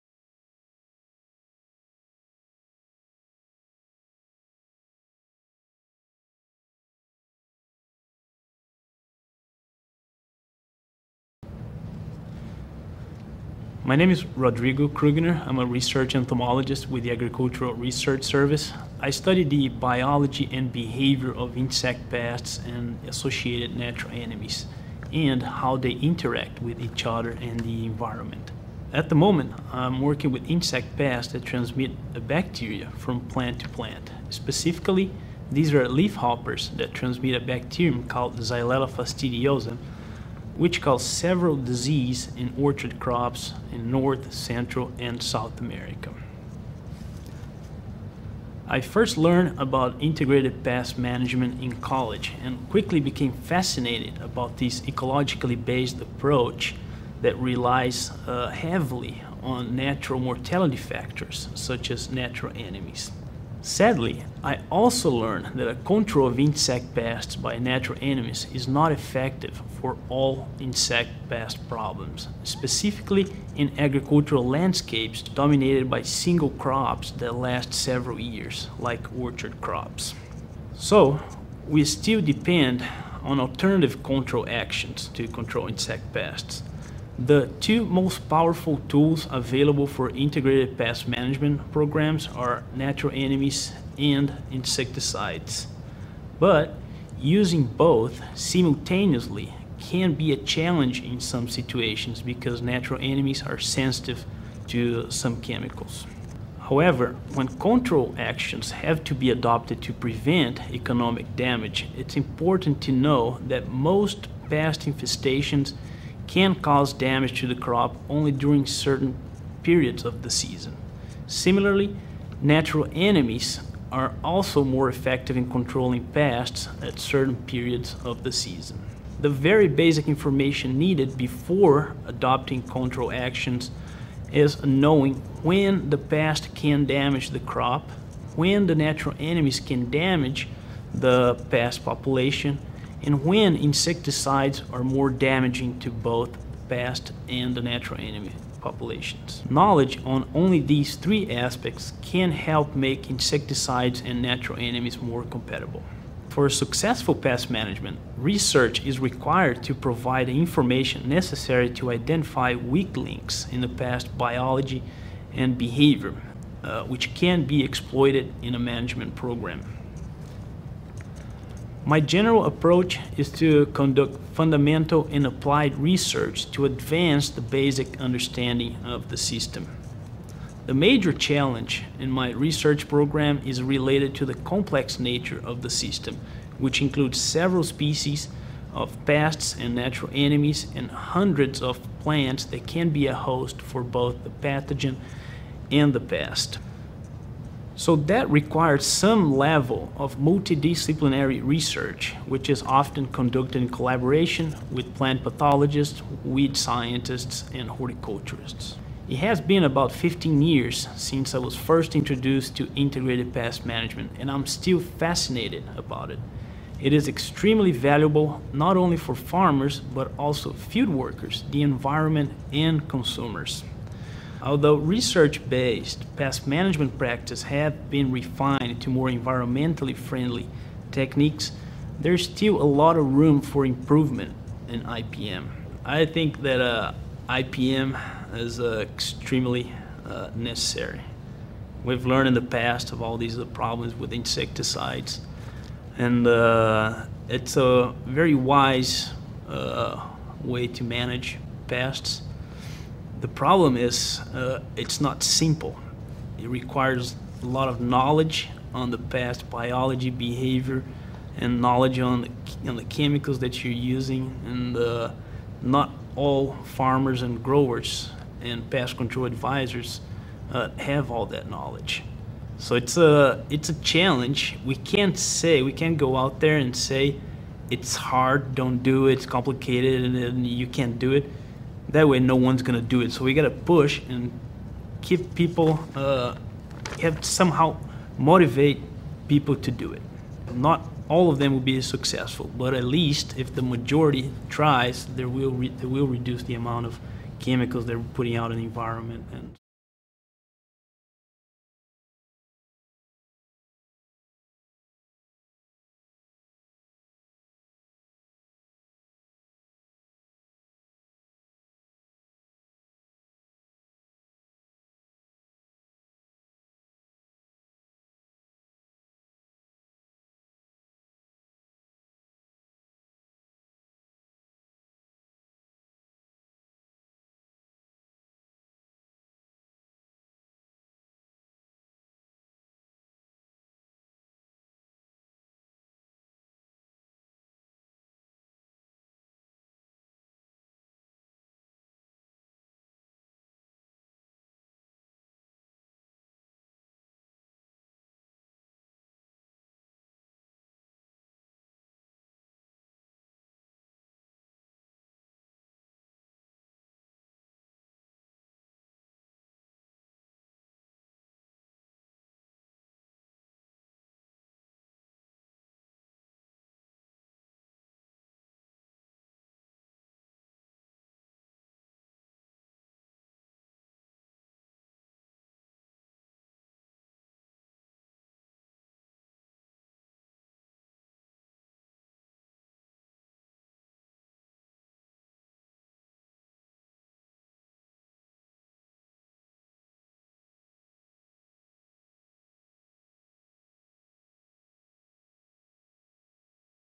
Rodrigo Krugner FINAL 12June2013 1
Dr. Rodrigo Krugner received his PhD in Entomology from the University of California (UC), Riverside, in 2009. He is a Research Entomologist employed by the USDA Agricultural Research Service and is located at the USDA San Joaquin Valley Agricultural Science Center, Parlier, CA. His research focuses on the glassy-winged sharpshooter (GWSS) and includes the identification and phenology of weed hosts of the plant pathogen Xylella fastidiosa that can serve as potential sources of inocula for infection of almond and grape plants, the identification and evaluation of stimuli that control GWSS reproductive biology and behavior; and the evaluation of potential pathogenicity of Xylella fastidiosa in olive plants.